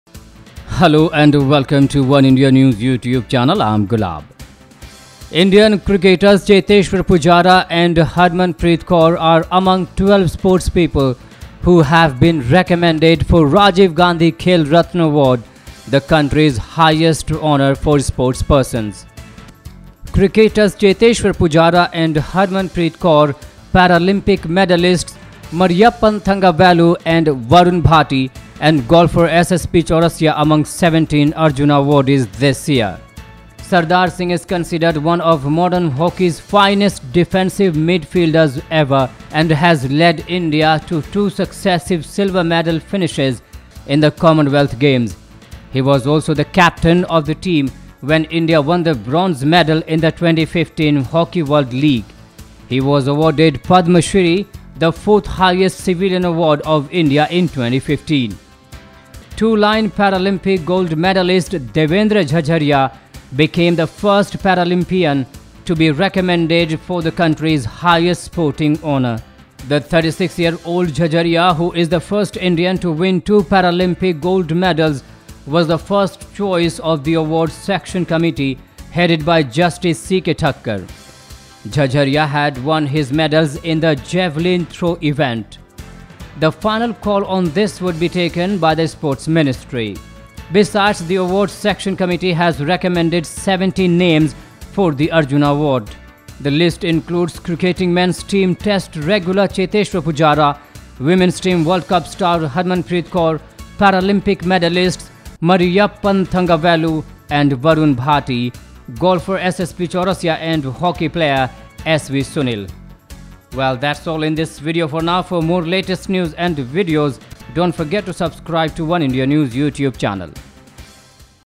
Cheteshwar Pujara and Harmanpreet recommended for the Arjuna Award in 2017 | Oneindia News
India cricketers Cheteshwar Pujara and Harmanpreet Kaur are among 12 sports people who have been recommended for the Arjuna Award in 2017. Devendra Jhajharia, who won a gold medal at the Paralympics last year and hockey star Sardar Singh have been recommended for the country's highest sporting honour, the Khel Ratna.